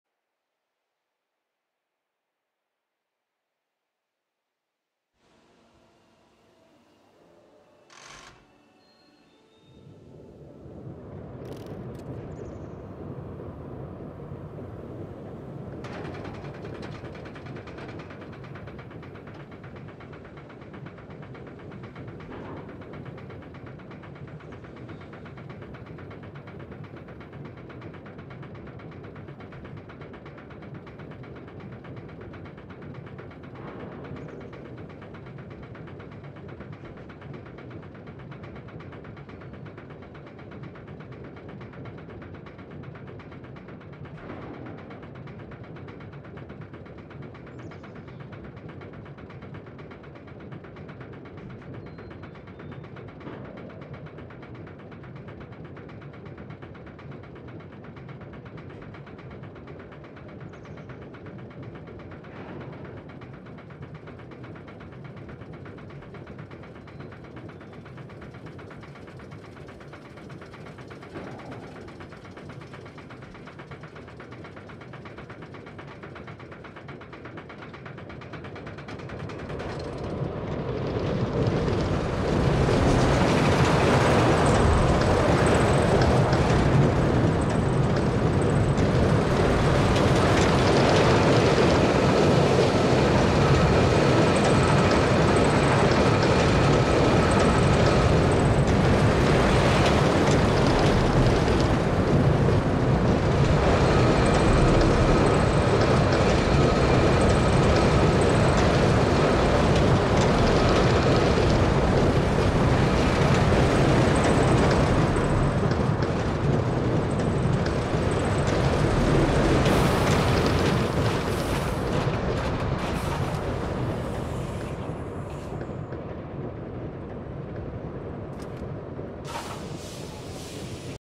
Revenge - Arrow Hyper Coaster
Revenge is an Arrow Hyper Coaster at SturgisPark New England.